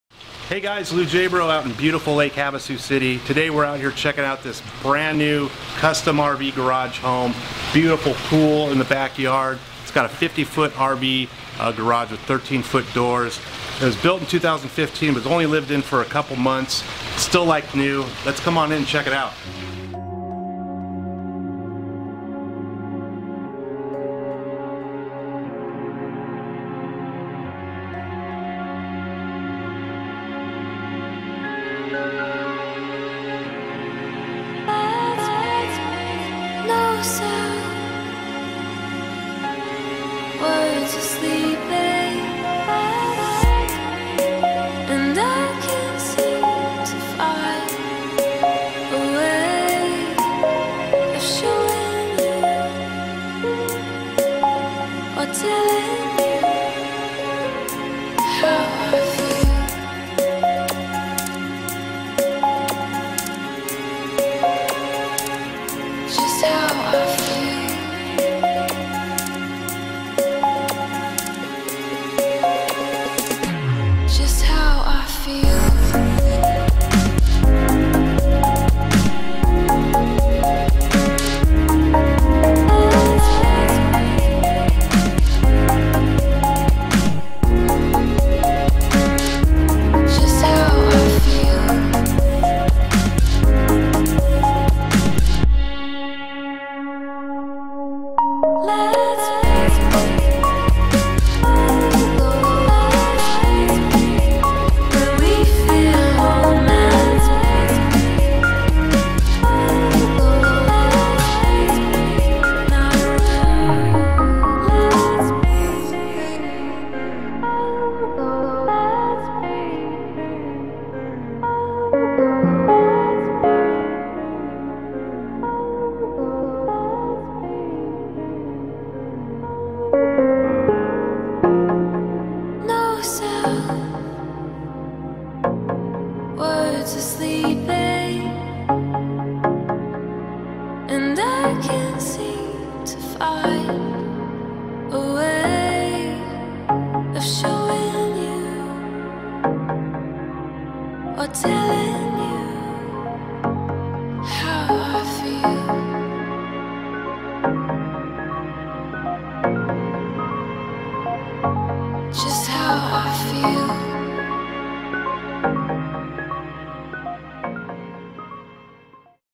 Lake Havasu Pool Home with RV Garage - 1794 Rainbow MLS 915845
for more information and current list price.

Check out this amazing, like new pool home with 50' RV garage built in 2015. Only lived in for a couple months and still like brand new!

Have questions and just want to talk to a real person?

Not what you are looking for? See ALL Lake Havasu RV garage homes for sale

Get caught up on the Lake Havasu real estate market